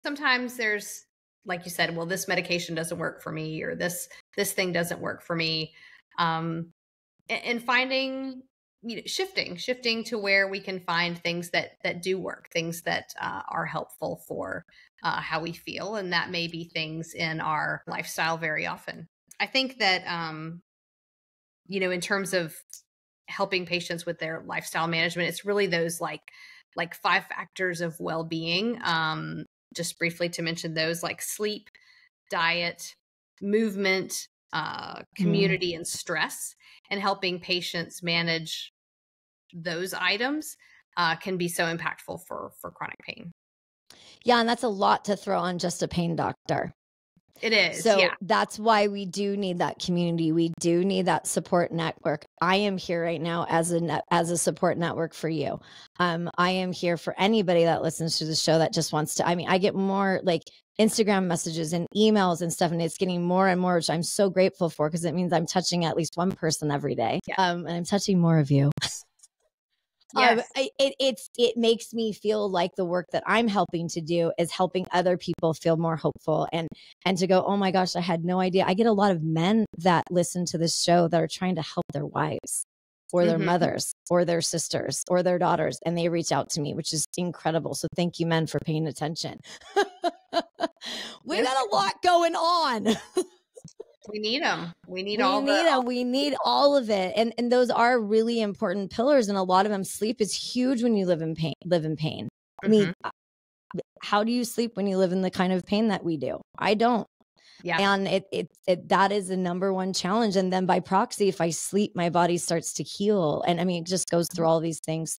The Power of Lifestyle and Community Support.
Are you struggling with chronic pain? 🩺✨

In this must-watch episode, Dr. Sarah Merritt and Lyndsay Soprano dive into the five key pillars of wellbeing that can transform your life: sleep, diet, movement, community, and stress management. Learn how these crucial factors can help you find relief and improve your quality of life. 🙌

Whether you're managing your own pain or supporting a loved one, this episode offers valuable insights and practical advice. Join our supportive community and take the first step towards a pain-free life!